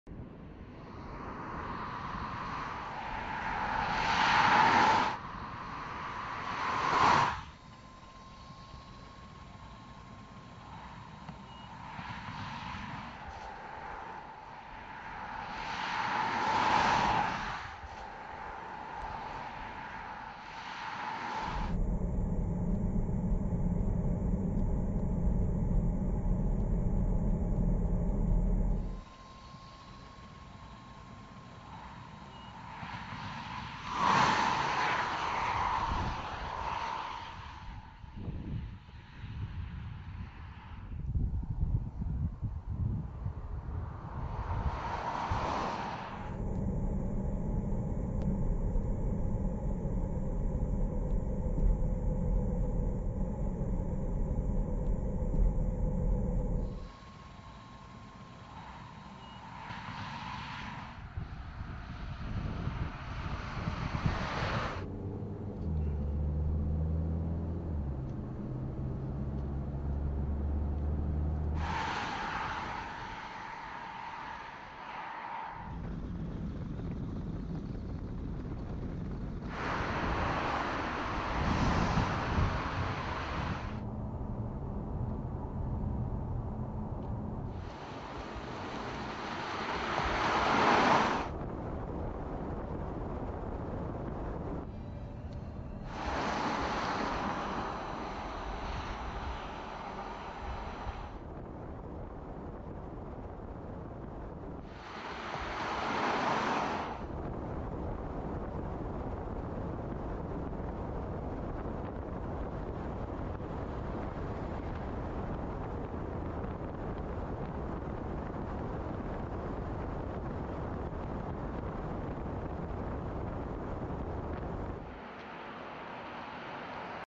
Volkswagen Arteon hybride rechargeable 2020
La Volkswagen Arteon embarque une propulsion hybride rechargeable eHybrid.

Le système eHybrid repose sur un moteur TSI de 156 ch (115 kW) renforcé par un moteur électrique de 115 ch (85 kW).